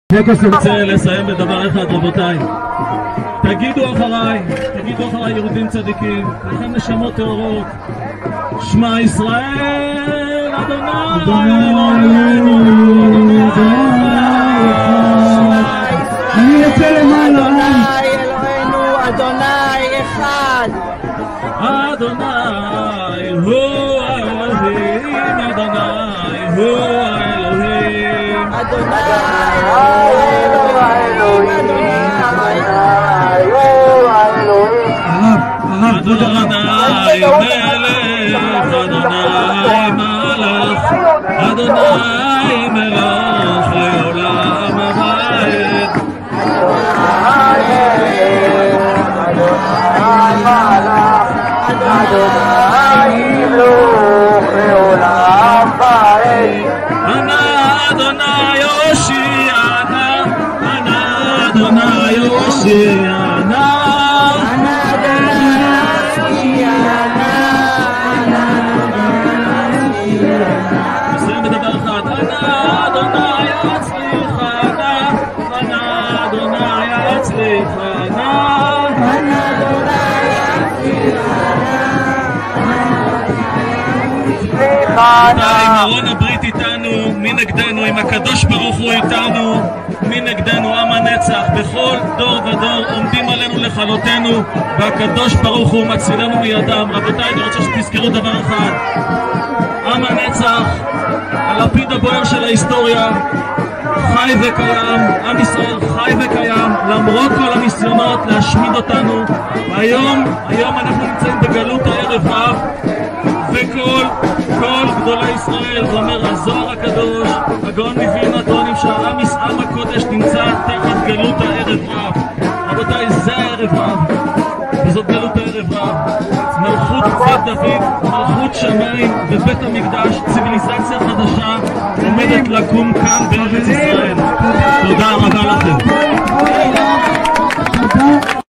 הרב דניאל עשור במסר לעם ישראל במחאה ליד הכנסת
הרב עשור ליד הכנסת במחאה, מעביר מסר לעם ישראל, את קריאת שמע ישראל אפשר לשים ביוטיוב - את המסר עצמו תוכלו לשמוע פה